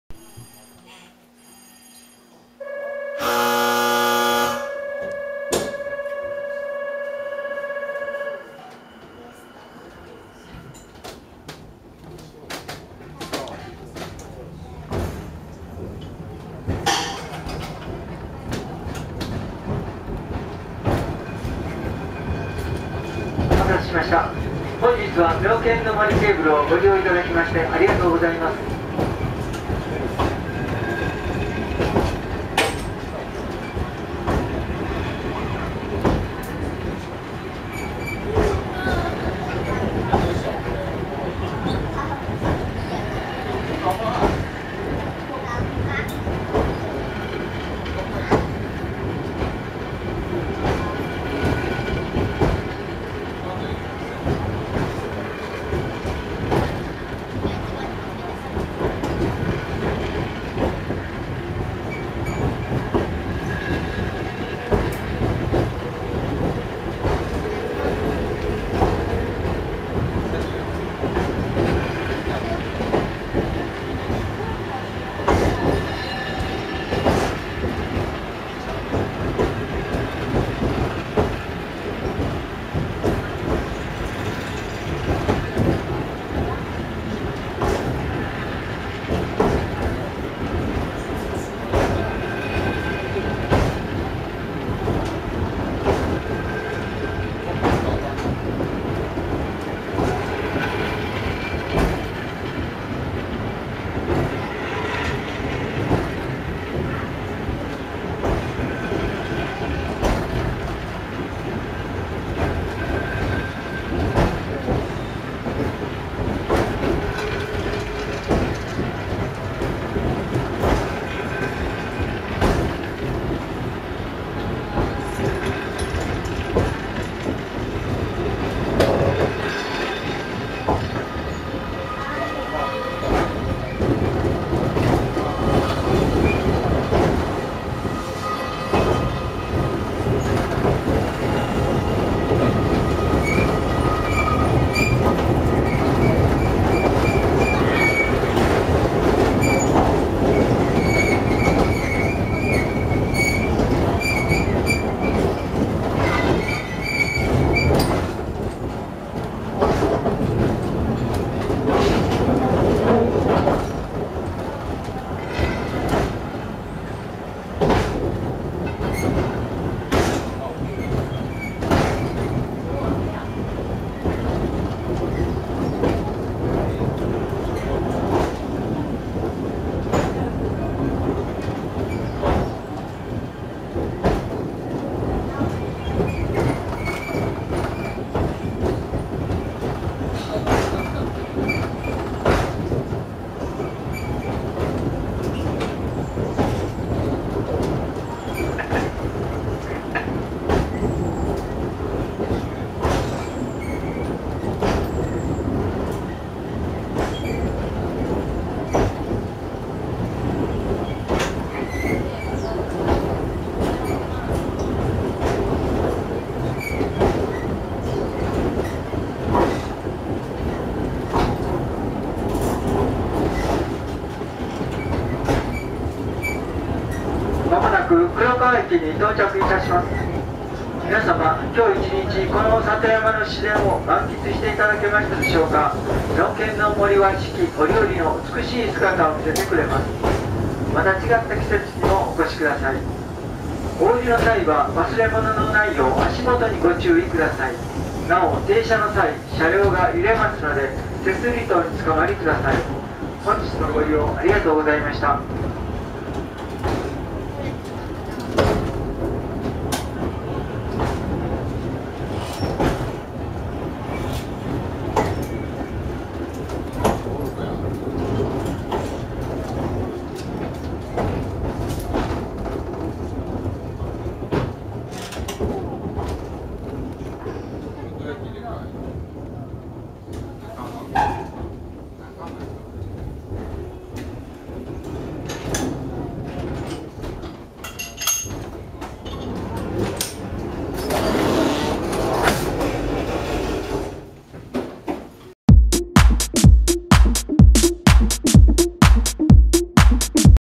2023年12月廃止 能勢電鉄 妙見ケーブル 車窓（ケーブル山上→黒川）
2023年12月4日をもって廃止された能勢電鉄の妙見ケーブルの在りし日の車窓を後方展望いたします。

去年は気づいたら動画を2本しか上げられなかったので今年はもう少し頑張って投稿していきたいと思っております。